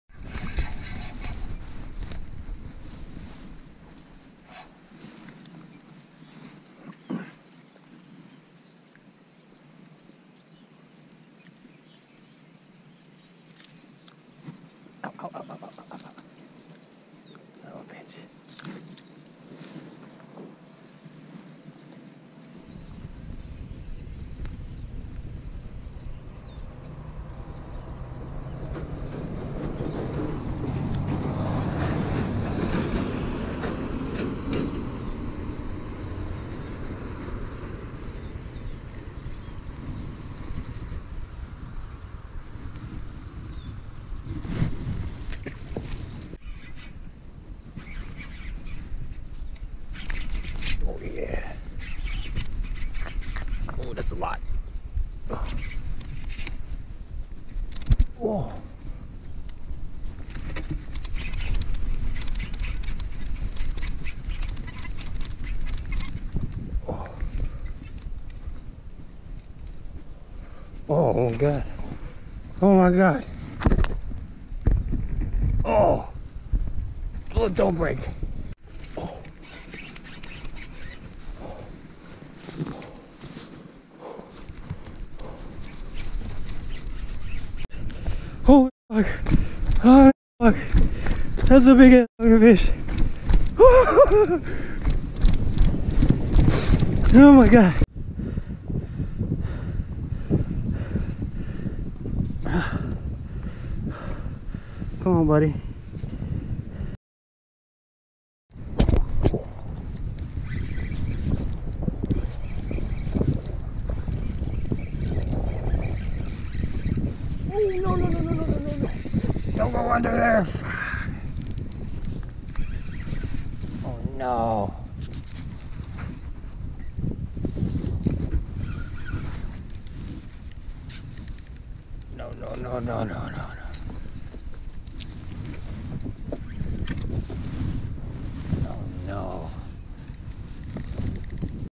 Fishing the bank. Caught one of the biggest bass!
Quick 4 hours of fishing the bank of the delta. Caught one of the bigger bass that I’ve caught using a punching rig.